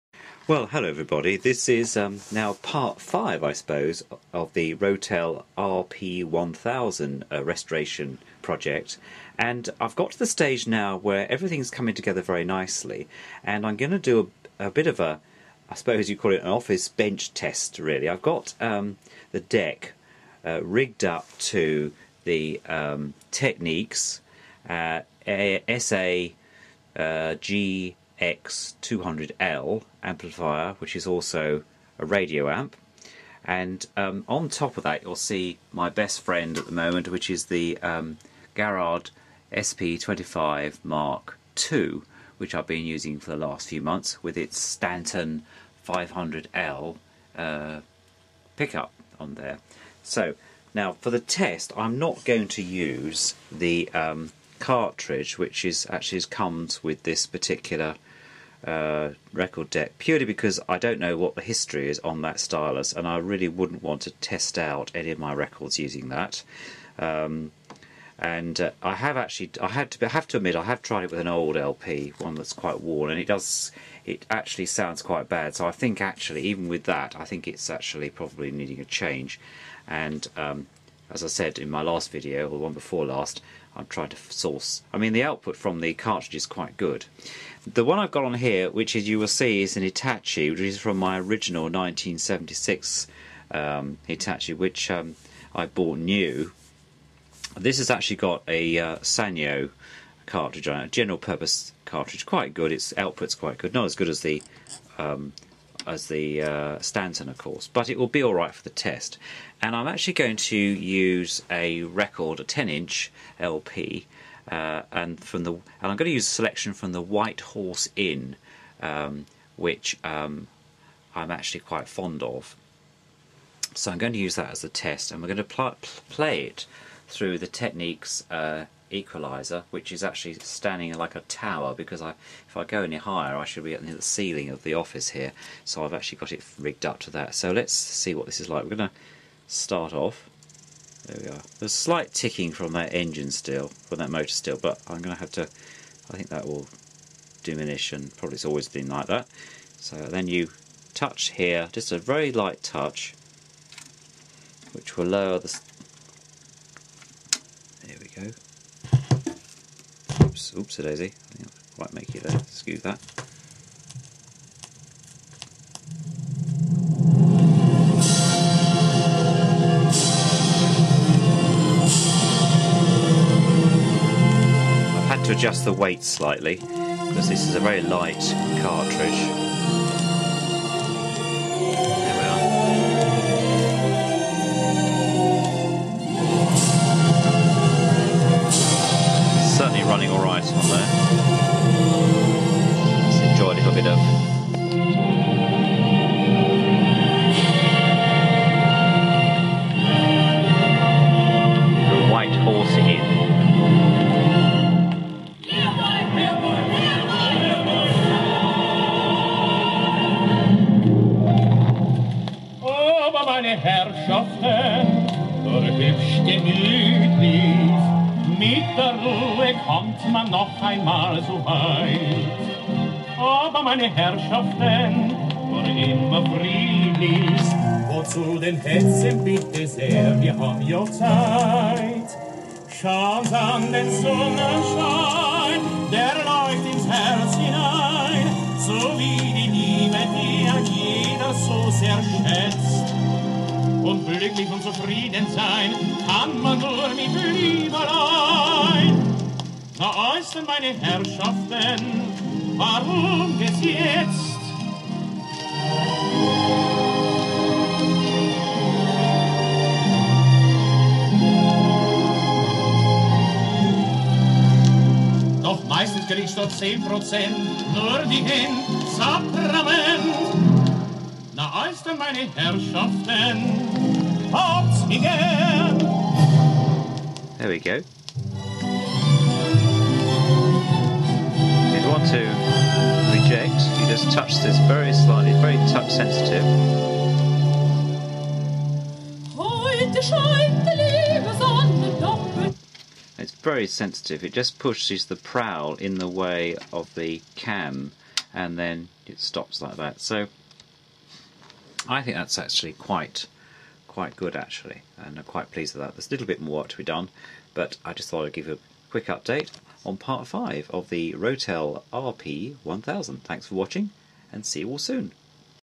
Rotel RP 1000 Pt 5.
Hi You Tubers,

This is part 5. for the Rotel record deck RP1000 - not as I mention on the video 18572 which is the series number.

This deck has been given to me by my colleague Philip at Avante Partnership.

Watch all parts of the programme and I look forward to receiving your comments.

Warm regards

Richard